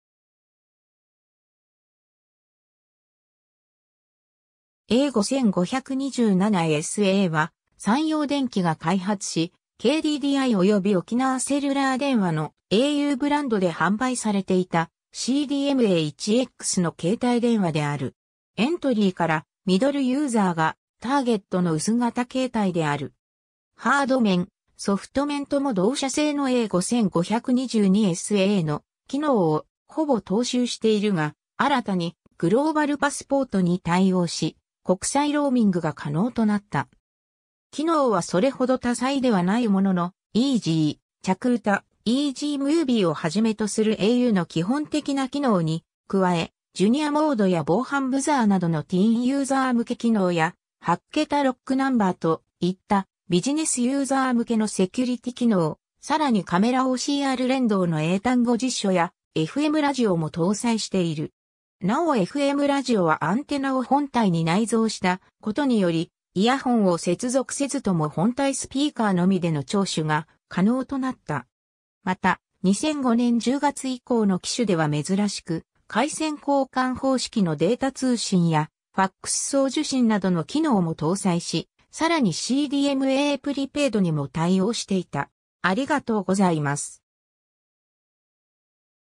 A5527SA
A5527SA（えーごーごーにーななえすえー）は、三洋電機（大阪、現・京セラ SANYOブランド）が開発し、KDDIおよび沖縄セルラー電話のauブランドで販売されていたCDMA 1X（現・au 3G）の携帯電話である。
 エントリーからミドルユーザーがターゲットの薄型携帯である。
ハード面、ソフト面とも同社製のA5522SAの機能をほぼ踏襲しているが、新たに「グローバルパスポート」（現・au世界サービス(CDMA)）に対応し、国際ローミングが可能となった。
 機能はそれほど多彩ではないものの、EZ「着うた」、EZムービーをはじめとするauの基本的な機能に加え、ジュニアモードや防犯ブザーなどのティーンユーザー向け機能や、8桁ロックナンバーといったビジネスユーザー向けのセキュリティ機能、更にカメラOCR連動の英単語辞書やFMラジオも搭載している。
なおFMラジオはアンテナを本体に内蔵したことにより、イヤホンを接続せずとも本体スピーカーのみでの聴取が可能となった（イヤホンを使用することで受信感度を向上させることもできる）。
 また、2005年10月以降の機種ではめずらしく回線交換方式（ASYNC）のデータ通信やFAX送受信などの機能も搭載し、さらにCDMAぷりペイドにも対応していた。